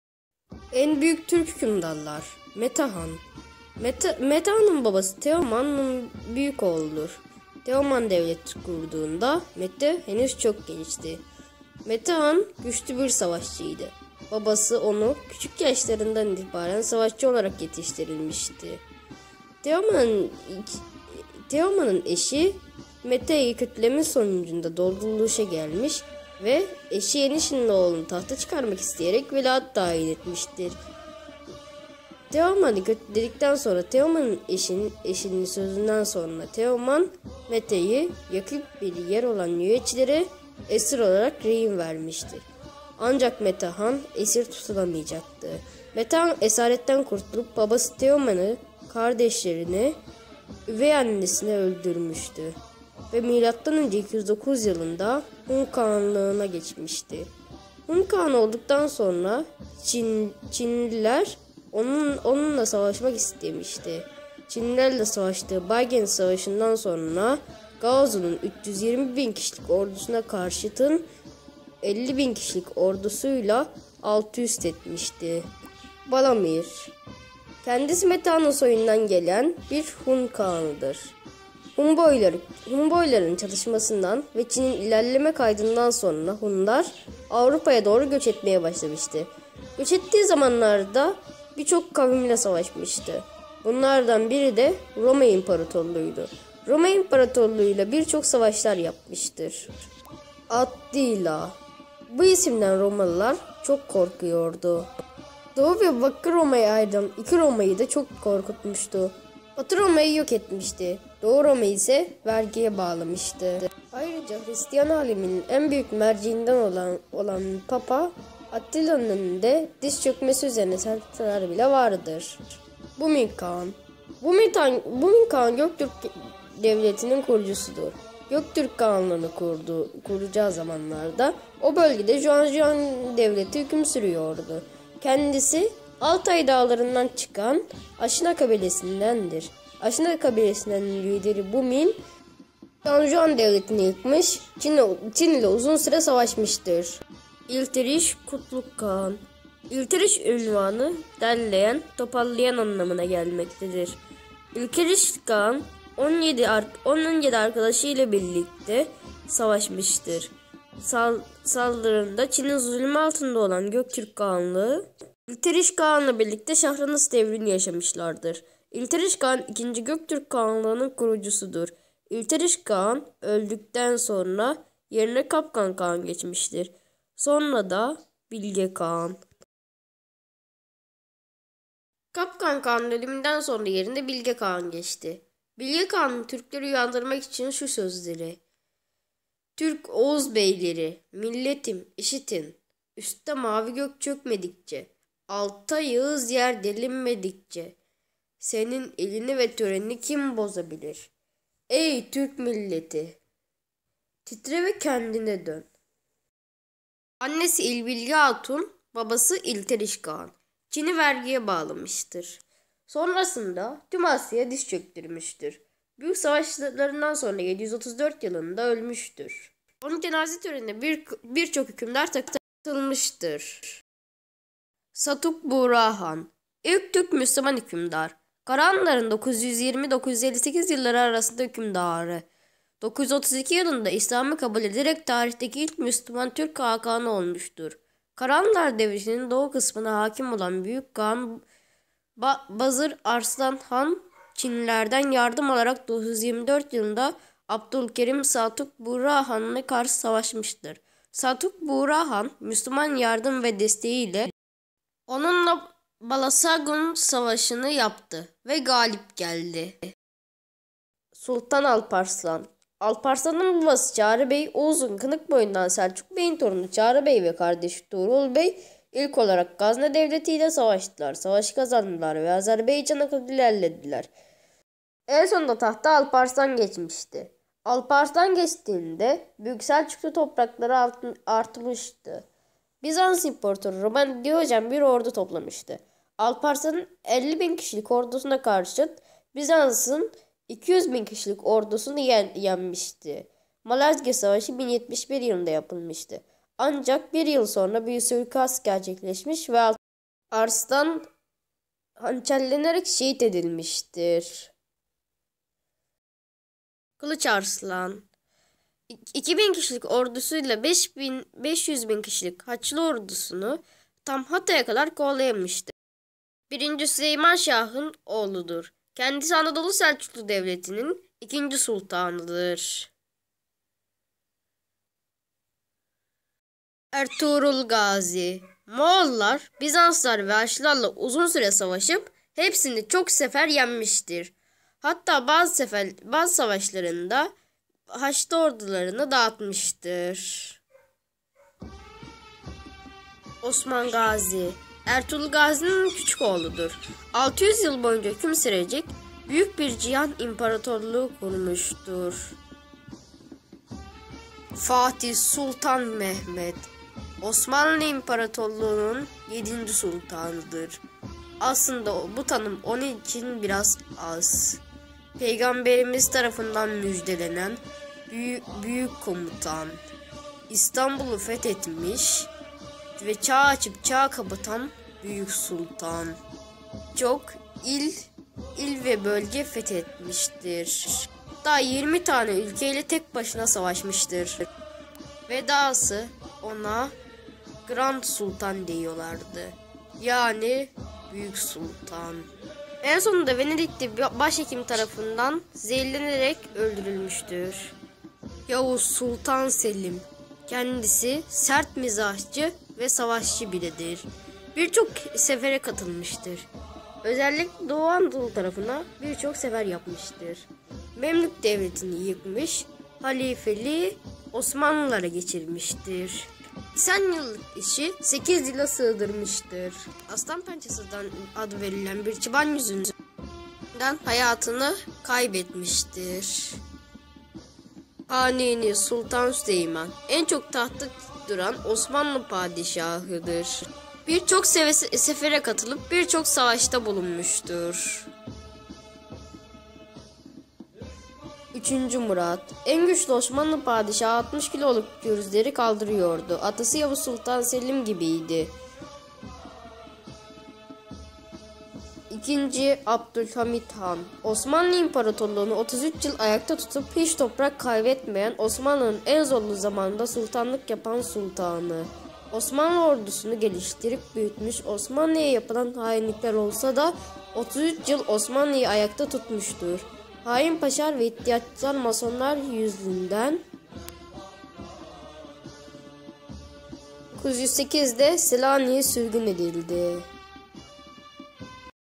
Dünyayı titreten 10 büyük türk hükümdar
En büyük türk hükümdarlar

Asya Hun imparatorluğu'nun kurucusu olan Teoman, oğlu Metehan'ın yerine üvey annesi Yenişi'nin oğlunu tahta çıkarmak istemiştir. Metehan üvey annesinin oyunları sonucunda Yuezhi’ler tarafından rehin alınmıştır.
Metehan, babası Teoman’ın Yuezhi topraklarına girmeden kaçarak kurtulmuştur. Bu başarısı ve Yuezhi'lerin mağlup edilmesinden dolayı Teoman Metehan’a 10000 kişilik bir ordu vermiştir. Metehan bu ordu ile öncelikle üvey annesini ve kardeşlerini, sonra da babasını öldürerek MÖ 209 yılında kağan olmuştur. Mete Önce Hunlardan toprak talebinde bulunan doğu komşuları tonghu üzerine yürüdü ve onları ağır bir yenilgiye uğrattı. Tanrı Dağları ile Gansu arasında yaşayan Yüeçilerin üzerine seferler düzenledi. MÖ 203'te Yueçi'yi mağlup ederek kendi toprağına kattı. Peteng Kalesinde kuşattı. Gaozu (Gao-Di) Mete'ye hediyeler göndererek kuzey eyaletlerini Hunlara bırakma ve yıllık vergi ödeme gibi bütün şartları kabul etti. Bunun üzerine imparatorun kuşatmadan çıkmasına müsade edildi.

Balamir
Kavimler göçünde Avrupaya giden Türkler Avrupa Hun Devletini kurdu. Balamir han Avrupaya doğru giderken yolculukta savaşlar yapmışdır.
Birçok kavimle savaşmışdır
Bumin kağan

Bumin Kağan (Eski Türkçe: Bumïn Kagan) veya İllig Kağan Pinyin: yīlì kěhàn, Wade-Giles: i-li k'o-han), Türk adıyla kurulmuş ilk devlet olan ilk Göktürk Kağanlığı'nın kurucusudur 552

İllig KağanHüküm süresiHaziran 552 – Mart 553
Mukan Kağan
Tapar Kağan
Böri Kağan Hanedan Aşina BabasıAşina Tuvu Doğum490 Ölüm Mart 553
Suyab Dini Tengricilik
Bumin Kağan Prenses Chang'le ile evlenerek Batı Vey hanedanı ile akrabalık kurdu. 552'de Huai-Huang'ın kuzeyinde Cücenlere ağ
ır ve ani bir darbe vurarak bu devleti ortadan kaldırdı. Daha sonra Ötüken merkezli Göktürk Kağanlığı'nı kurdu. Devletinin batı kanadının yönetimini küçük kardeşi İstemi Yabgu'ya verdi.

680 yılında Aşina soyundan gelen Kutluk, akıl hocası Bilge Tonyukuk ile birlikte Çin'e isyan etti. M.S. 681 yılında "On yedi arkadaşı" ile birlikte Göktürk Kağanlığı'nı yeniden kurmak için teşebbüse geçti, Ötüken başkent olmak üzere, Kağanlığı örgütledi. Çin'e olmak üzere kırk yedi sefer tertip eden İlteriş Kutlug Kağan, yirmisine bizzât katıldı. Hepsinde başarılı olup, hiç yenilmediği rivayet edilir.
Ölünce yerine kardeşi Kapgan Kağan geçmiştir.

Bilge Kağan
(Eski Türkçe:
, Bilge qaγan[1], Pinyian kehan Tengriteg Тengride bolmuş Türk Bilge Kagan[2]:Tanrı gibi gökte olmuş Türk Bilge Kağanı İkinci Göktürk kağanı
(Tanrı Gibi Gökte Olmuş Türk Bilge Kağanı)Hüküm süresi Şubat 717 – 25 Kasım 734 Çocukları Tengri Tigin
Yollıg Tigin
Syüan
Ta-lo Hatun Hanedan Aşina Babası İlteriş Kağan Annesi İlbilge Hatun Doğum 683
Çugay-Ḳuz Dağı Ölüm 25 Kasım 734
Ötüken Defin 22 Haziran735 DiniTengricilik

İlk türk müslüman hükümdar. Karahanlıların 920-958 yılları arasındaki hükümdarı, 932 yılında İslam'ı kabul ederek, tarihteki ilk Müslüman Türk Hakanı olmuş. Karahanlılar Devletinin doğu kısmına hakim olan Büyük Kağan Bazır Arslan Han Çinlilerden yardım alarak 924 yılında Abdülkerim Satuk Buğra Han'a karşı savaş açtı. Satuk Buğra Han Müslümanların yardım ve desteğiyle, onunla Balasagun Savaşını yaptı ve galip geldi.

gazneli mahmut

Alparslan

Ertuğrul gazi
Haclılarla bizanslılarla ve moğollarla uzun süre savaşdı.

Osman gazi
Üç kıtaya hükmeticek kutlu bir imparatorluk kurucak ve bu imparatorluk 600 yıl dünyaya hükmedicekti.

Orhan gazi

1. murat hüdavendigar

Yıldırım beyazıd

Mehmet Çelebi

2. Murat

Fatih sultan mehmet

2. Beyazıt

Yavuz sultan selim

Kanuni sultan süleyman

3. Murat
En güçlü Osmanlı padişahı 60 kiloluk gürzleri kaldırabiliyordu. Atası Yavuz sultan selim gibiydi.

2. Abdülhamit han
Osmanlı imparatorluğunu 33 yıl ayakta tutup hiç toprak kaybetmiyen Osmanlının en zorlu zamanında sultanlık yapan sultan. Osmanlı ordusunu gelişdirip büyütmüş Osmanlıya yapılan hainlikler olsada 33 yıl osmanlıyı ayakta tutmuşdur. Hain paşalar ve İddiyatçılar masonlar yüzünden 1908 de selaniğe sürgün edildi.

Sonraki video: Gazne devletinin kuruluşu gazneli Mahmut